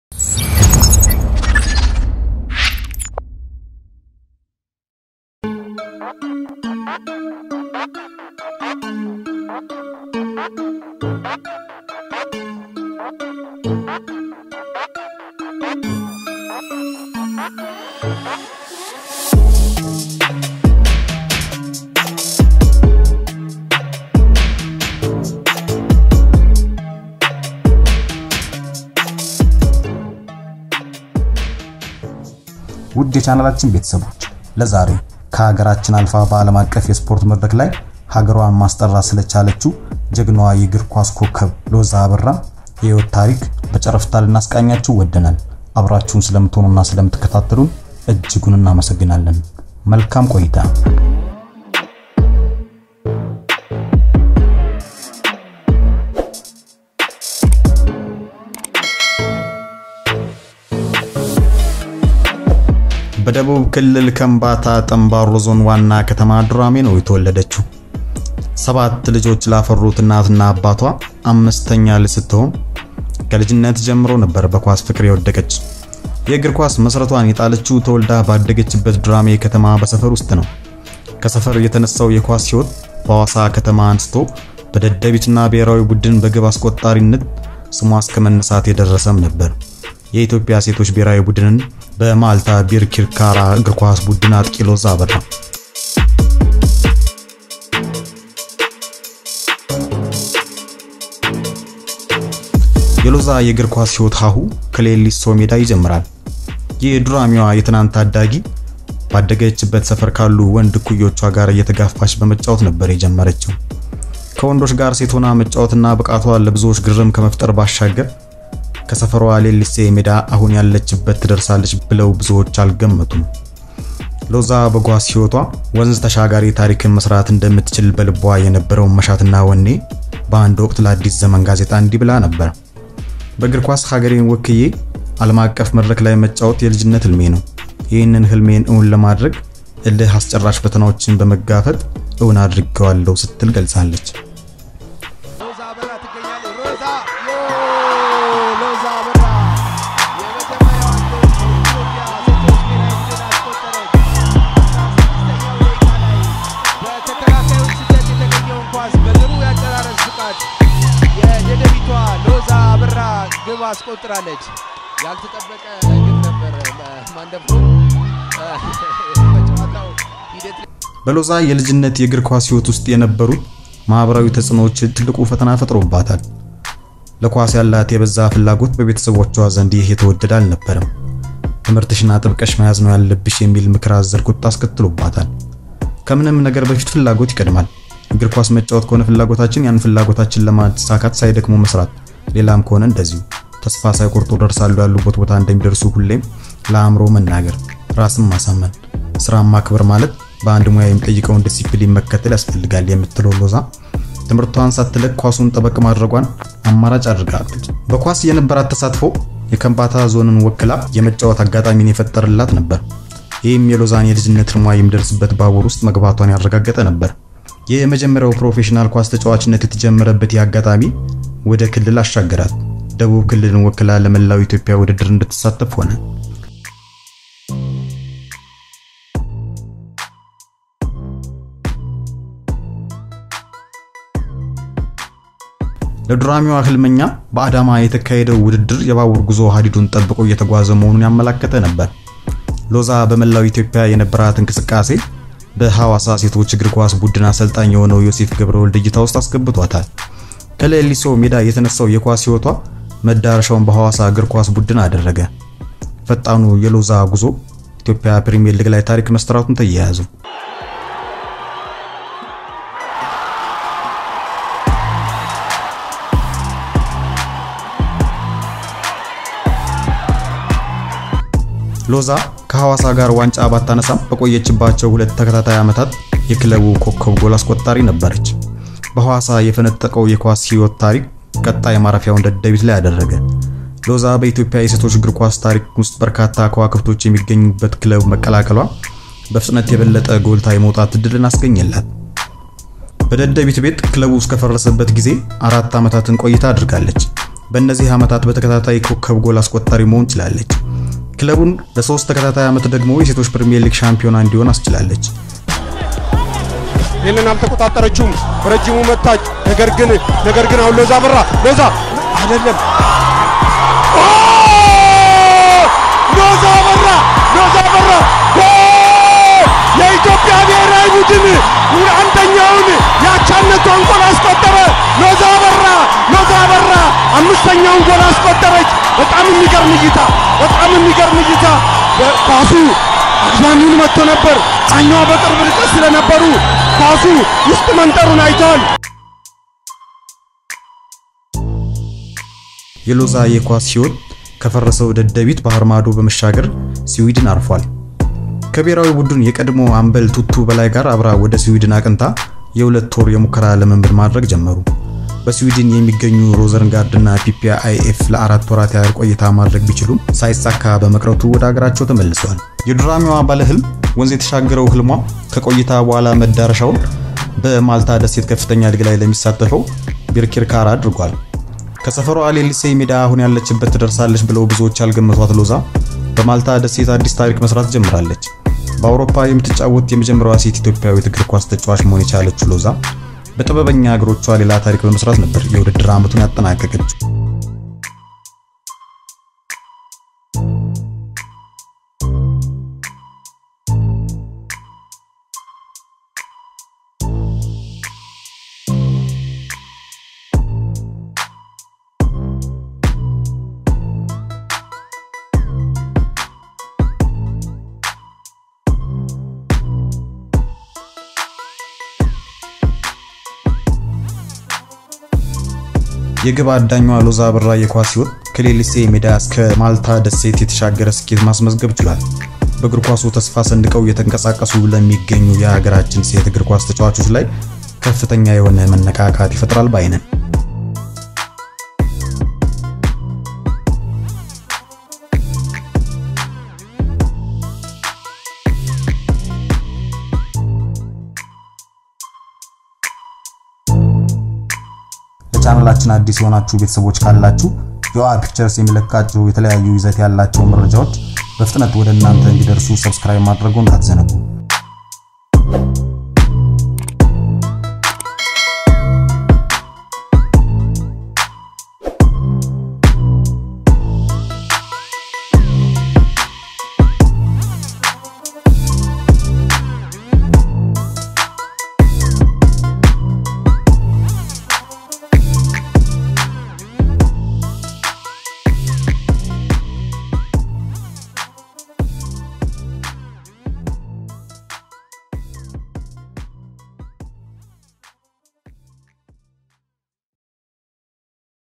ሎዛ አበራ_የዱራሜዋ የአውሮፓ ሊግ ኮከብ | Loza Abera_ STAR Woman From Duramie Ethiopia_to Malta Leagues
YoA Pictures| Loza Abera| Malta League| Birkirkira| Ethiopia || Biography